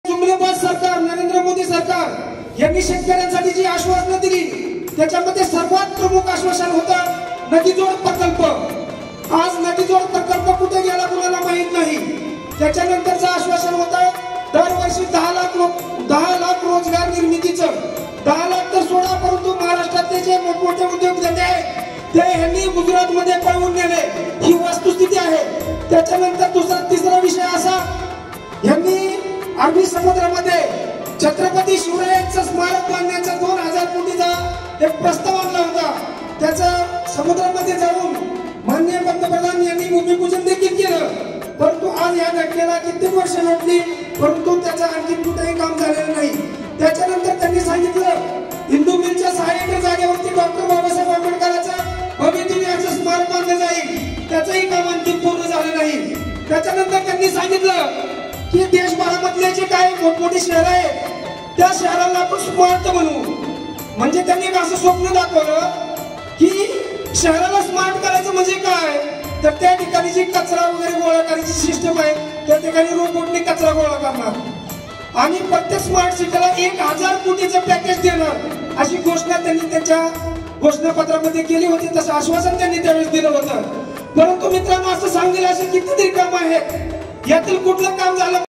भाजपने दिलेल्या या आश्वासनांचे काय झाले ? गणेश घुगे
महाविकास आघाडीच्या निर्धार मेळाव्यात बोलताना गणेश घुगे यांनी नरेंद्र मोदी सरकारने दिलेल्या आश्वासनावर प्रश्न विचारले...